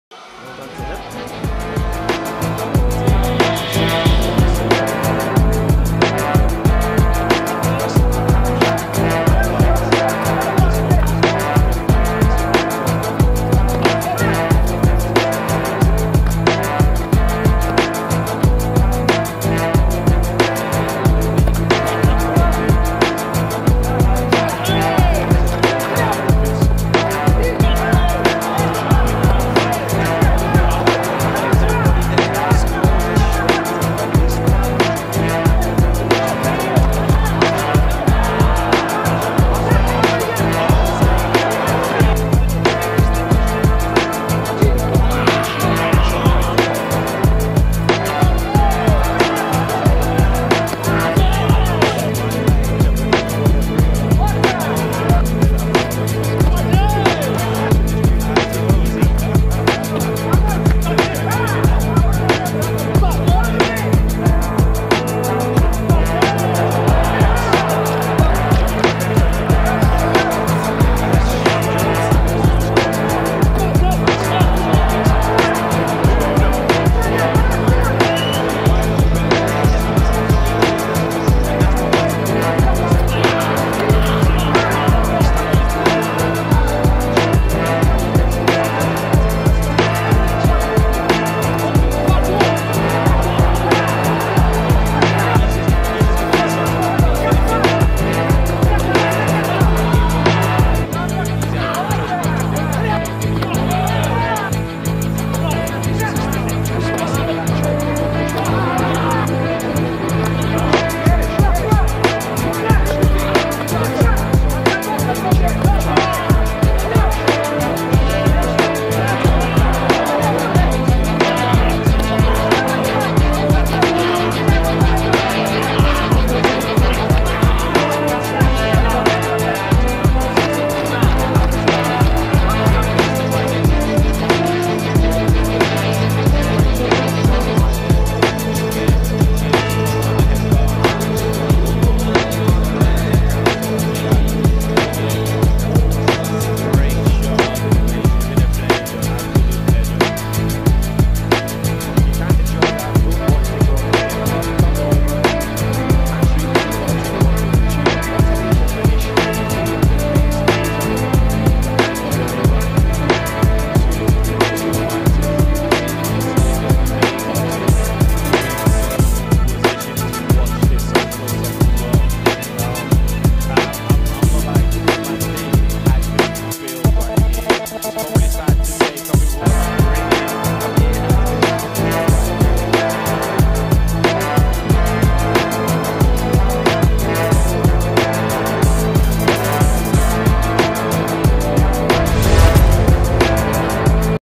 Taekwondo Grand Prix - Chiba 2019 - Maksim Khramtcov (RUS) VS Beigi Harchegani (AZE), final -80kg
Taekwondo Grand Prix - Chiba 2019 - Maksim Khramtcov (RUS) VS Beigi Harchegani (AZE), final -80kg [28-38]

Highlights Video edited from @worldtaekwondo channel